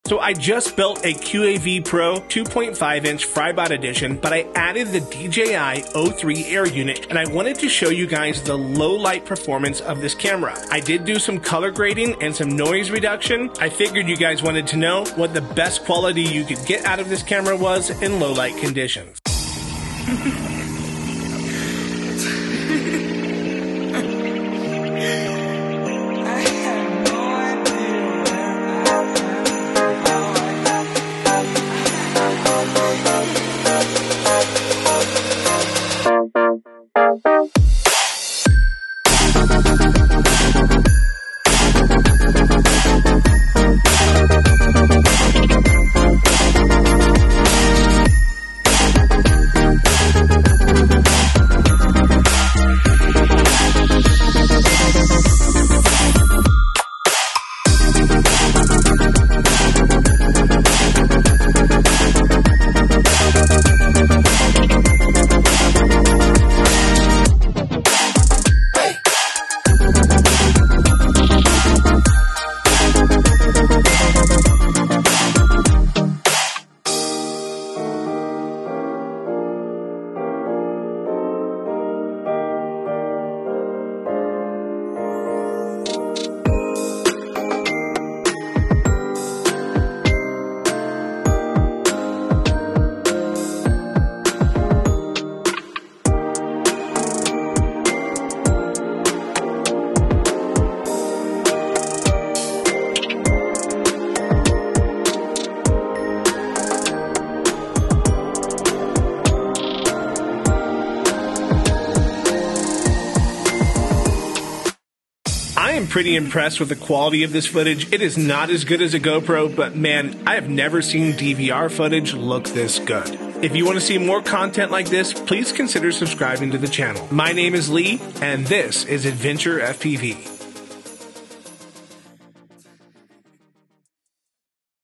DJI O3 Air Unit (Low Light Flight Footage)
I was working on a client job and thought this would be a great spot to test my new QAV-CINE 2.5 Build with the new DJI O3 Air unit. The client work was completed with the Shendrone Squirt and GoPro Hero 11 Mini but I could not resist the opportunity to test the onboard low-light recording of the O3. It did a decent job after some color grading and noise reduction using Neat Video. The quality of the feed is also really nice on the Goggles 2. One thing I do not like about this system is only being able to see the "final" look through the goggles. With a separate FPV cam, I can adjust the brightness way up to see better when flying and then expose my action cam to the scene.

⚙️⚙️⚙️ (Gear Featured in this Video) ⚙️⚙️⚙️
🥽 DJI Goggles
On Amazon
🎮 DJI FPV Remote
On Amazon
📷 GoPro Hero10
On Amazon